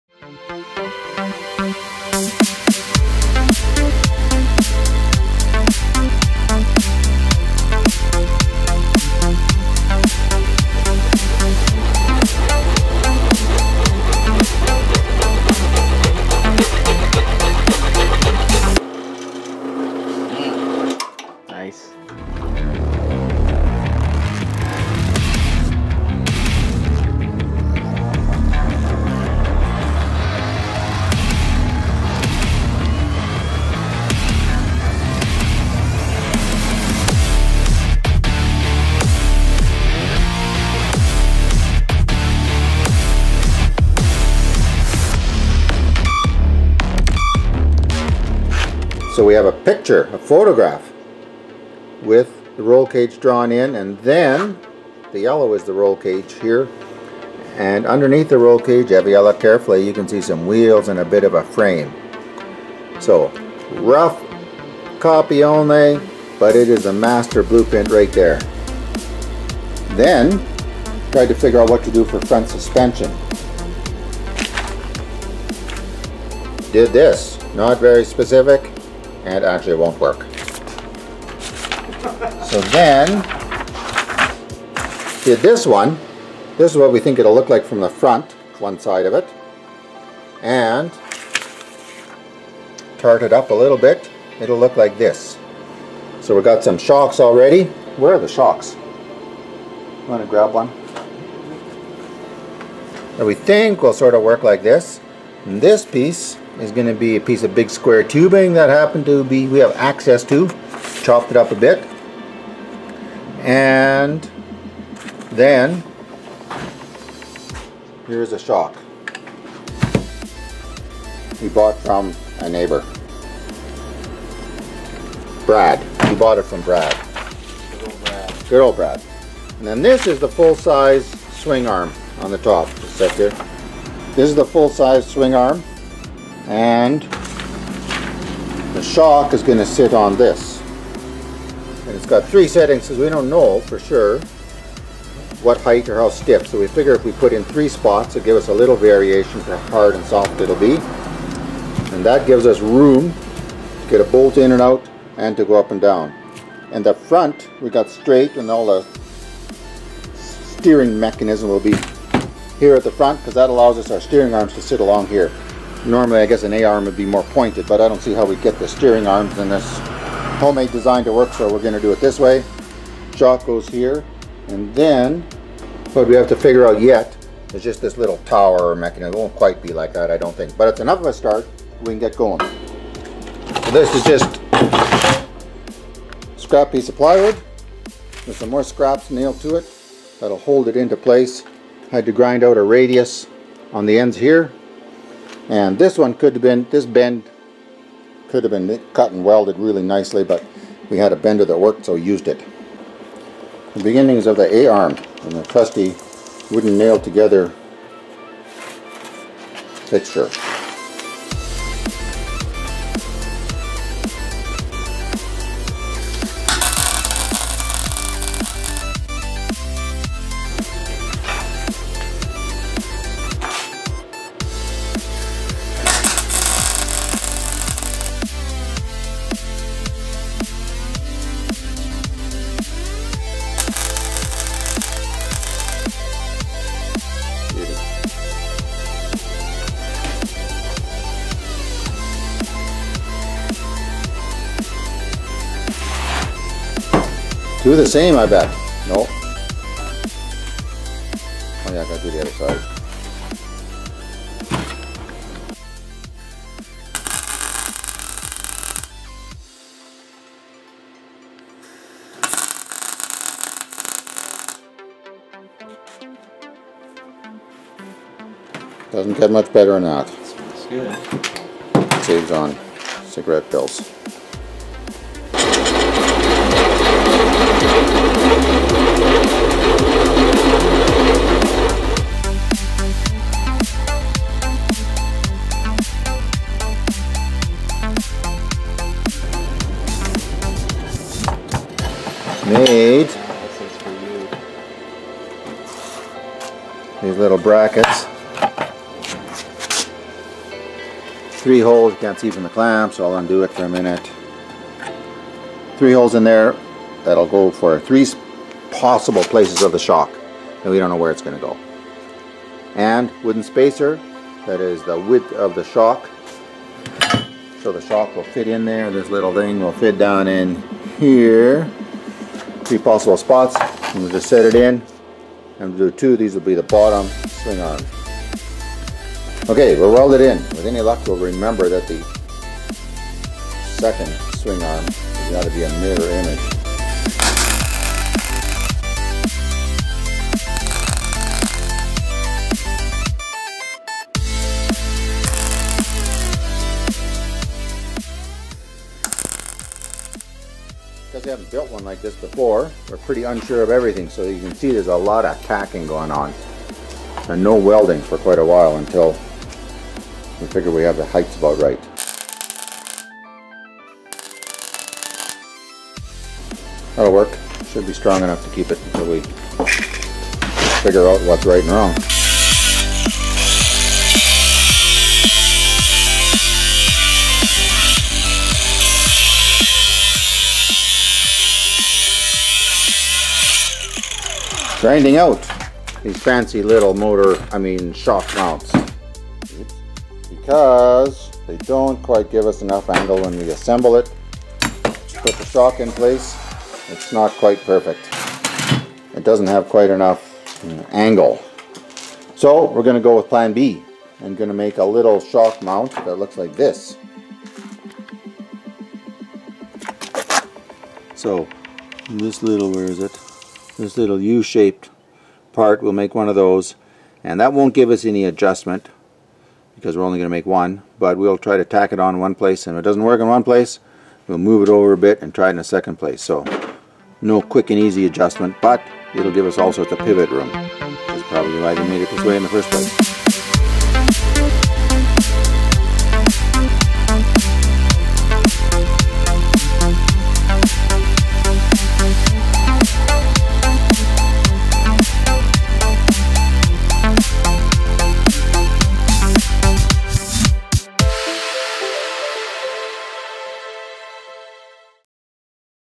Go-Kart Build Episode #3 (Front Suspension)
Making the A-Arms.  Part 1 of 2